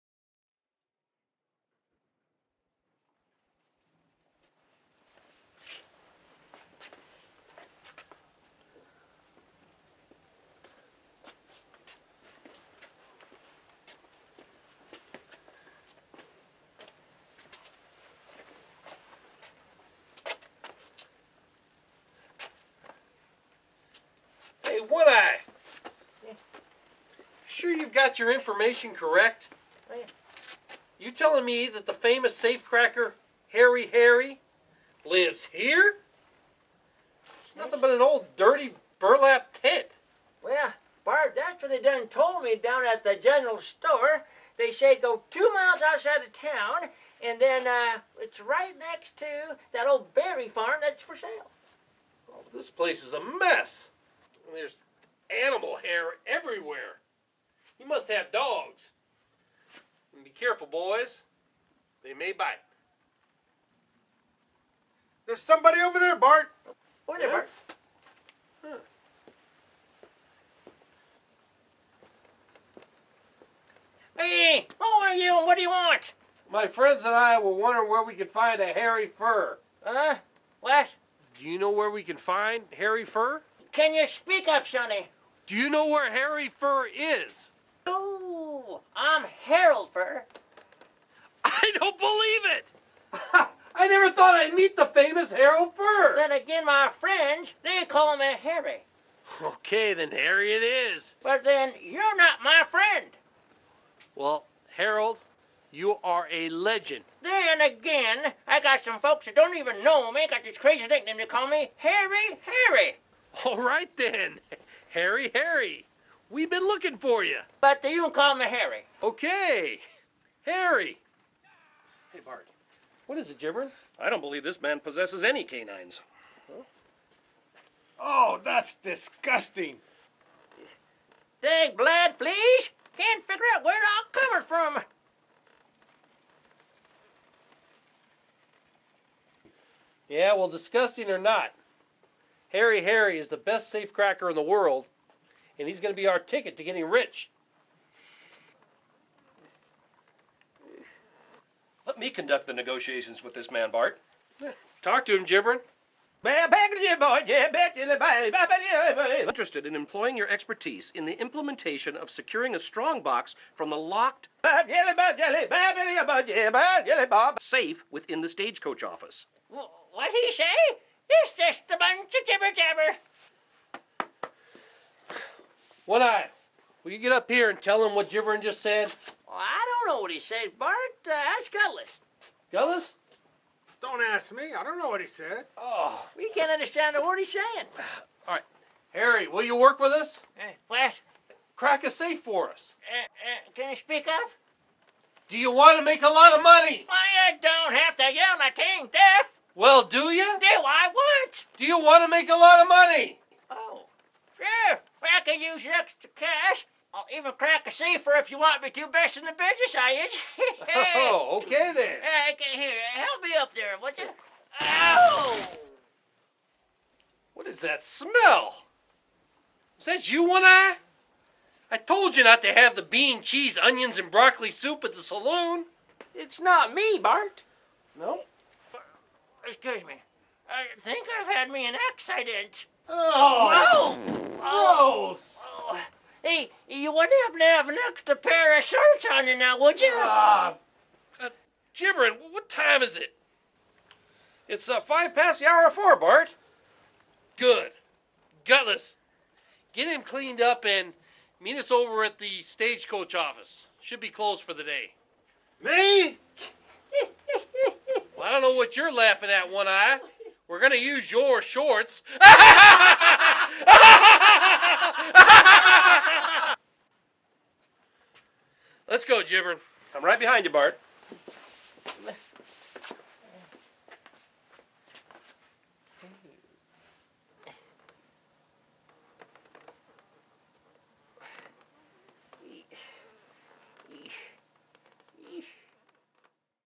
Great Safe Burglary - Part 3 - Tent Scene
The Chronicles of Black Bart - Scene 3 from Episode #2, "The Great Safe Burglary." Synopsis: Black Bart is planning on stealing the stagecoach strongbox from the Stage office.   But first, he has to find the infamous Hairy Harry, world renowned safe cracker.  This is a western themed spinoff series from "The Adventures of Black Bart in the world of the magical couch" which featured, "The Slouch In The Couch Childrens Corner Band."